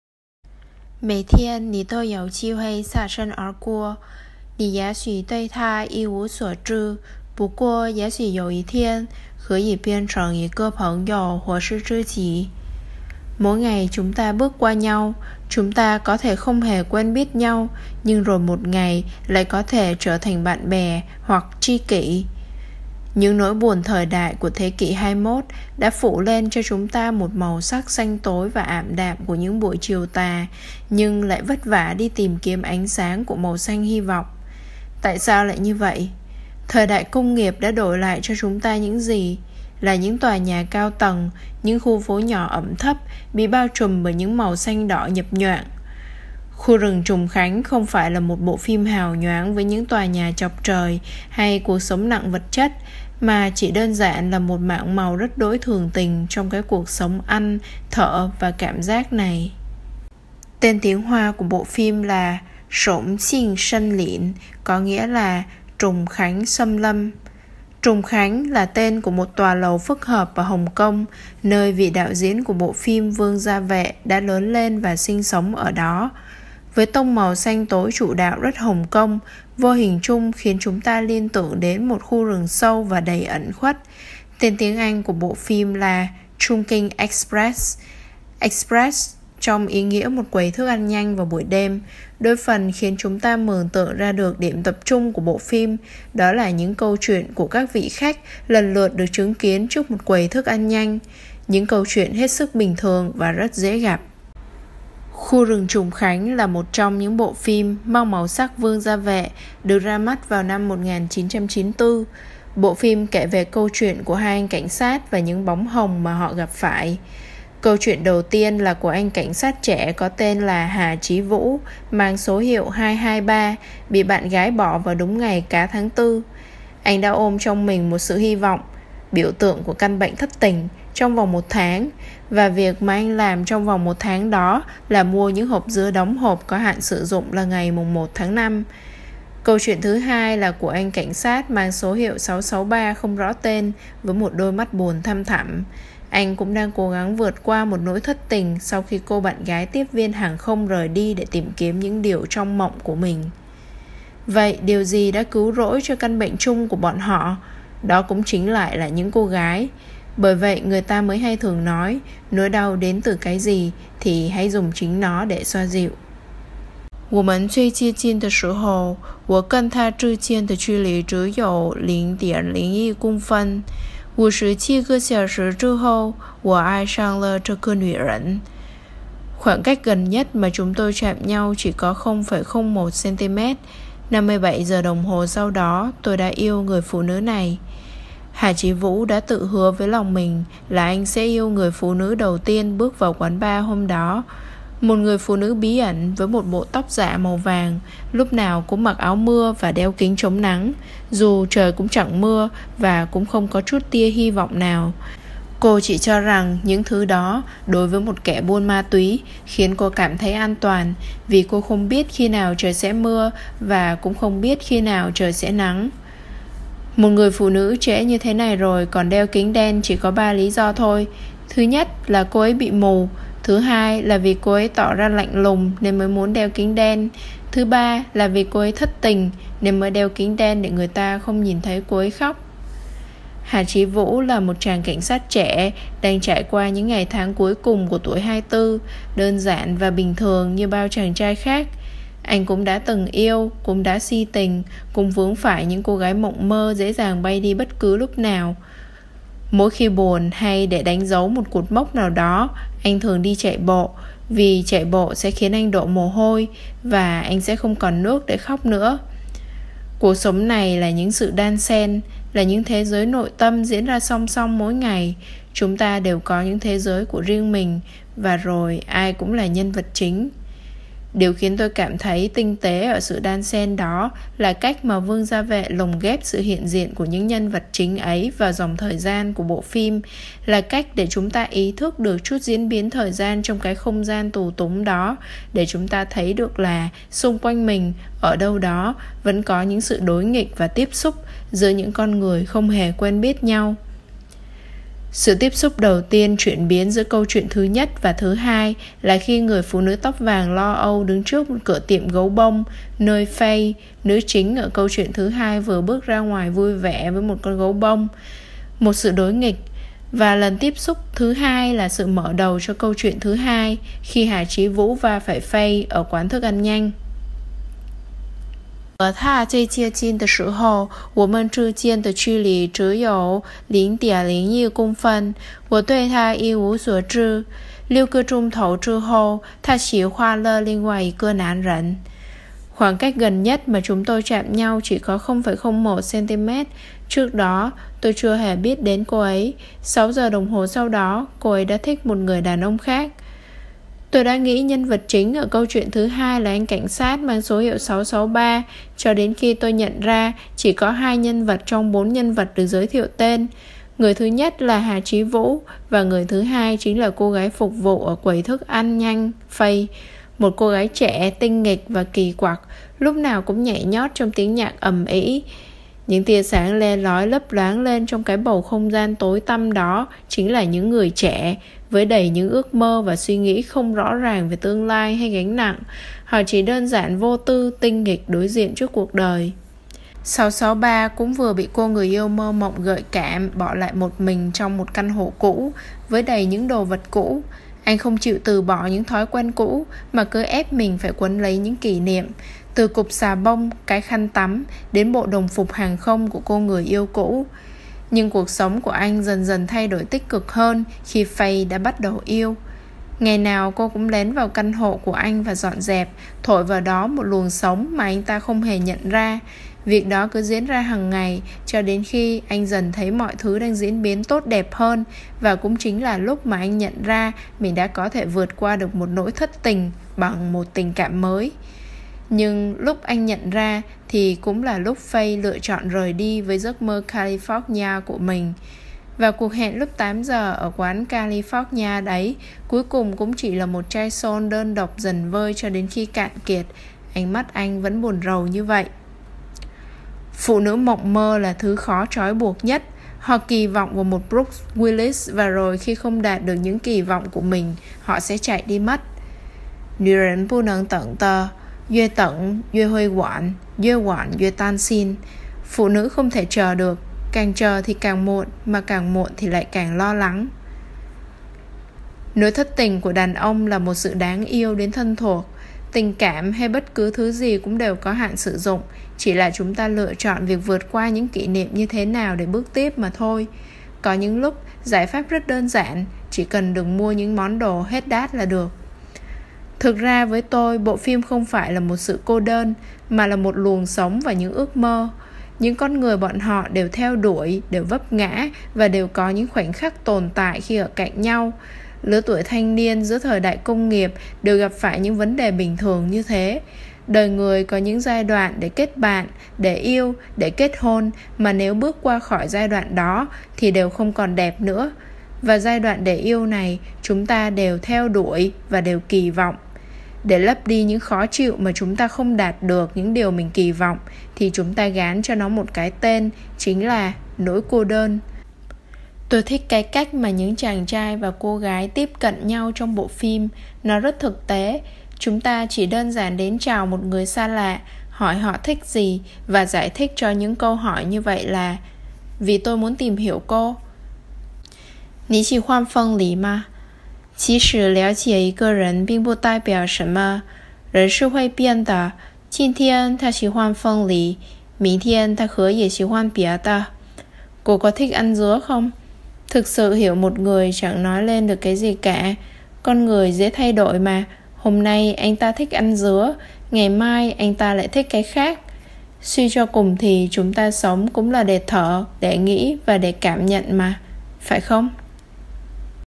Khu rừng Trùng Khánh (Vương Gia Vệ) - Một mảng màu rất đỗi thường tình
Một review về bộ phim ChungKing Express của đạo diễn Vương Gia Vệ do Minh Họa thực hiện. Cảm ơn các bạn đã lắng nghe!